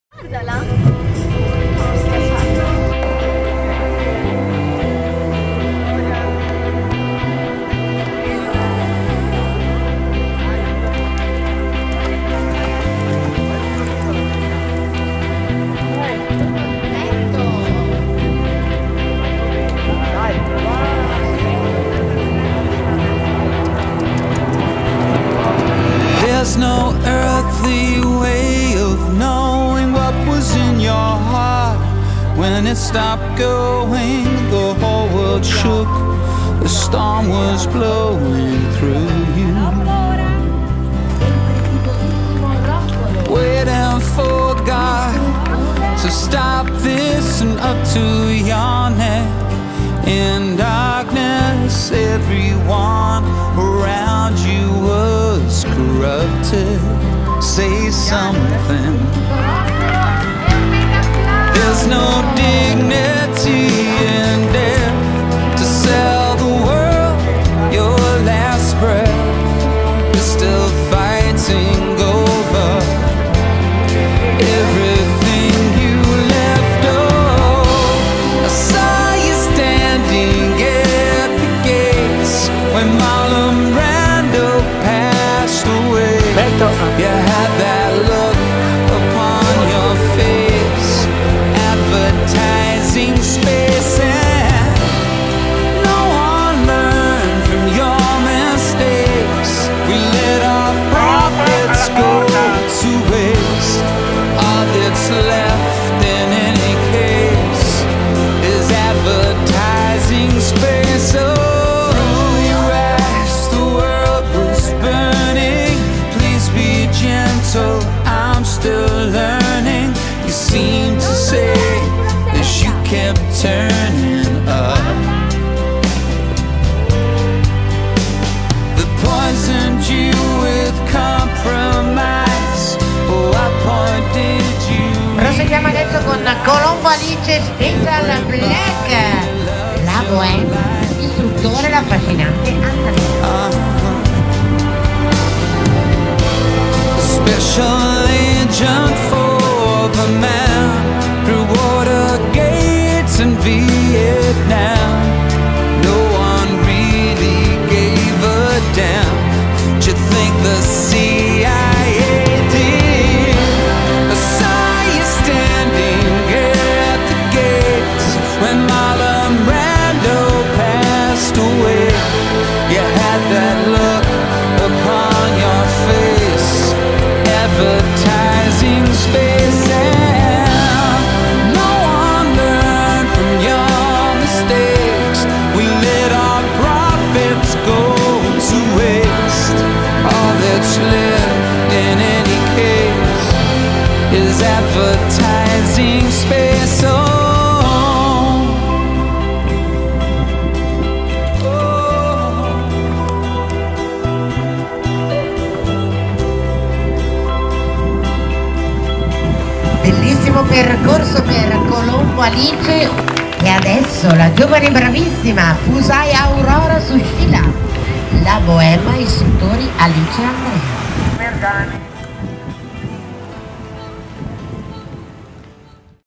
Carobbio 28 Agosto 2011 - Campionato ACSI Lombardia - Special Black debutta con Alice
SPECIAL BLACK al debutto in jumping A60